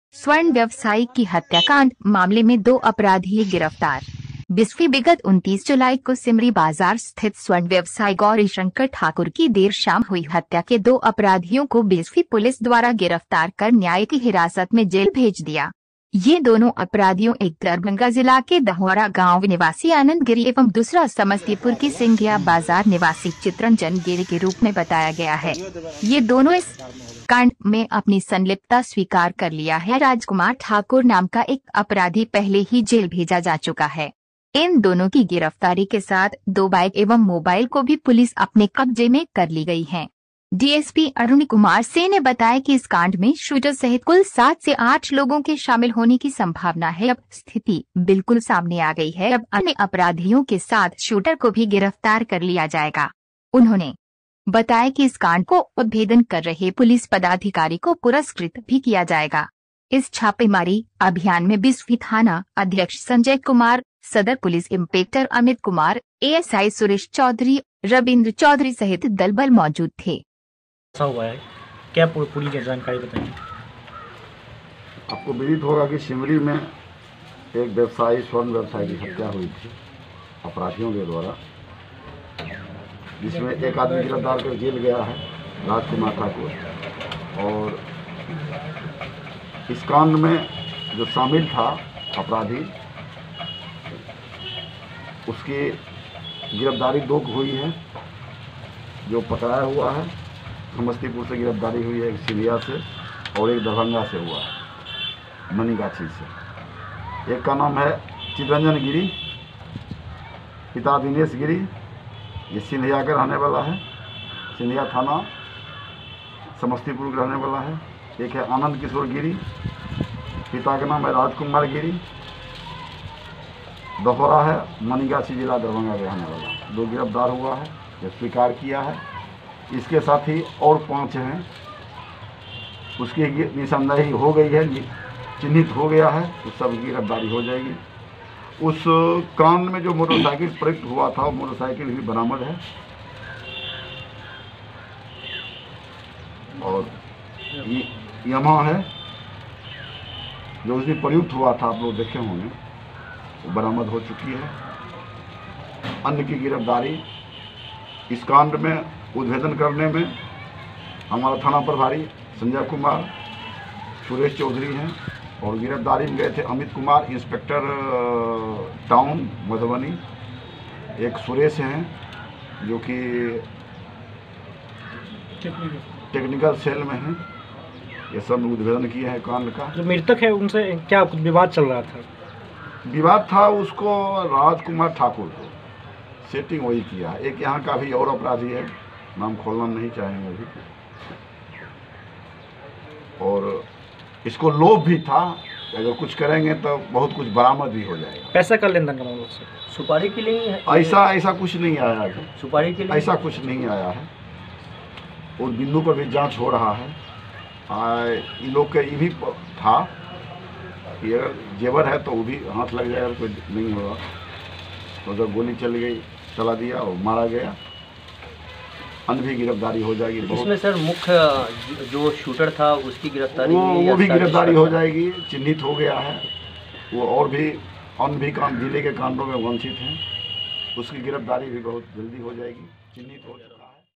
##स्वर्ण व्यवसाई की हत्या कांड मामले में दो आरोपी गिरफतार
आप सभी लोग देख रहे हैं पांडव न्यूज लाइव

स्वर्ण व्यवसाई की हत्या कांड मामले में दो अपराधी गिरफ्तार।

बिस्फी बिगत 29 जुलाई को सिमरी बाजार स्थित स्वर्ण व्यवसायी गौरीशंकर ठाकुर की देर शाम हुई हत्या के दो अपराधियों को बिस्फी पुलिस द्वारा गिरफ्तार कर न्यायिक हिरासत में जेल भेज दिया। ये दोनों अपराधियों एक दरभंगा जिला के दहौरा गांव निवासी आनन्द गिरि एवं दुसरा समस्तीपुर के सिंघिया बाजार निवासी चितरंजन गिरि के रूप में बताया गया है। ये दोनों इस कांड में अपनी संलिप्तता स्वीकार कर लिया है।राजकुमार ठाकुर नाम का एक अपराधी पहले ही जेल भेजा जा चुका है। इन दोनों की गिरफ्तारी के साथ दो बाइक एवं मोबाइल को भी पुलिस अपने कब्जे में कर ली गई हैं। डीएसपी अरुण कुमार सिंह ने बताया कि इस कांड में शूटर सहित कुल सात से आठ लोगों के शामिल होने की संभावना है।अब स्थिति बिल्कुल सामने आ गई है अब अन्य अपराधियों के साथ शूटर को भी गिरफ्तार कर लिया जाएगा। उन्होंने बताया कि इस कांड को उद्भेदन कर रहे पुलिस पदाधिकारी को पुरस्कृत भी किया जायेगा। इस छापेमारी अभियान में बिस्फी थाना अध्यक्ष संजय कुमार, सदर पुलिस इंपेक्टर अमित कुमार, एएसआई सुरेश चौधरी, रबिंद्र चौधरी सहित दल बल मौजूद थे।##